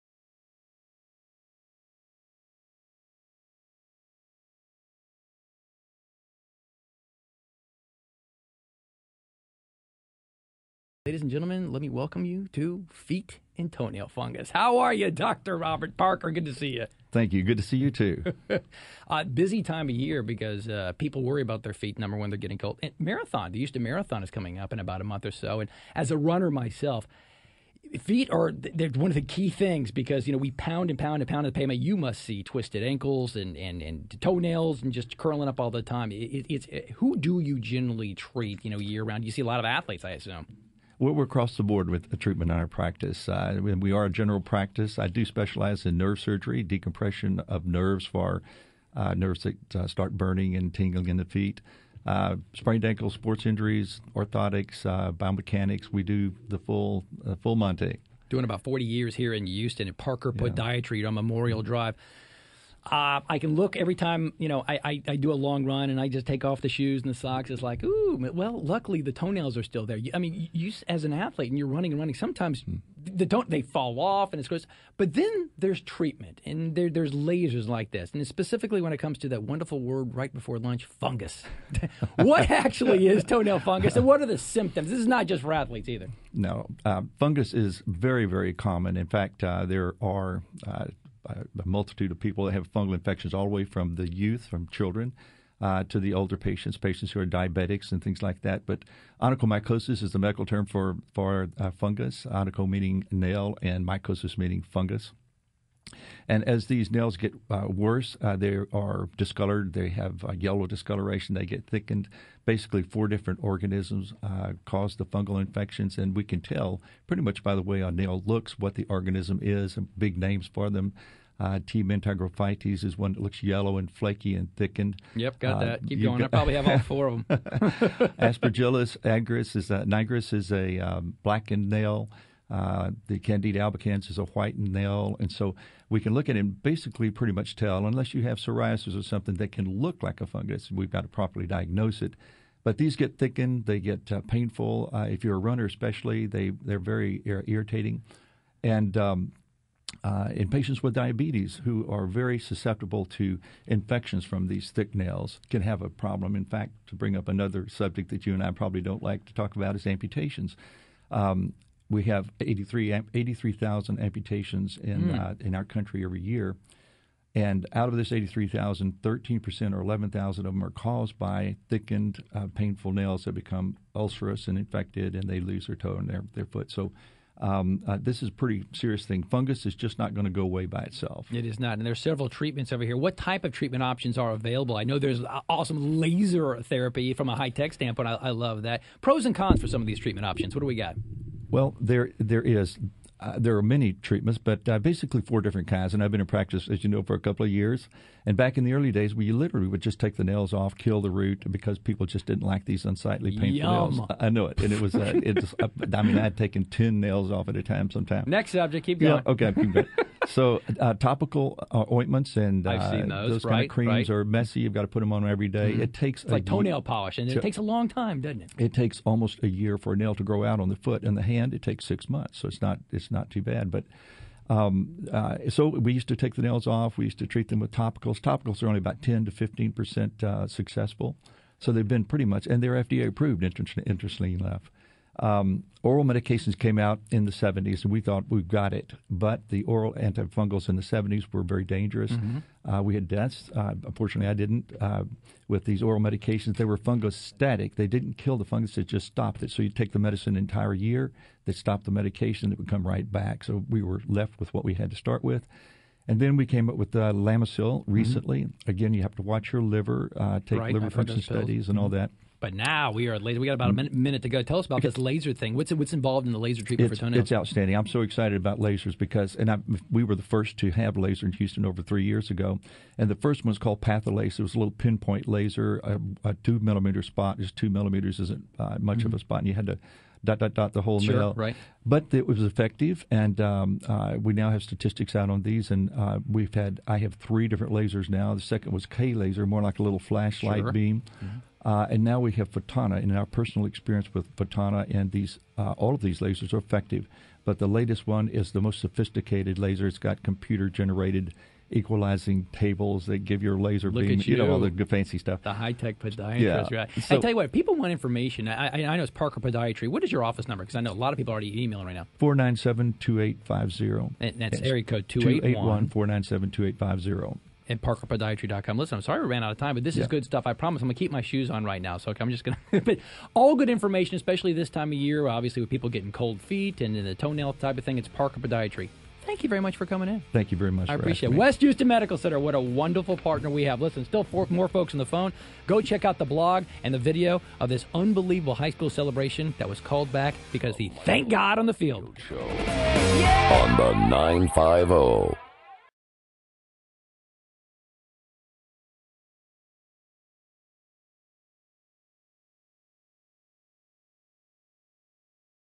The High Tech Podiatrist | Dr. Robert Parker and Michael Garfield Radio Interview
Dr. Robert Parker a podiatrist in Houston, TX talks with Michael Garfield about fungal toenails and the various ways to treat the problem.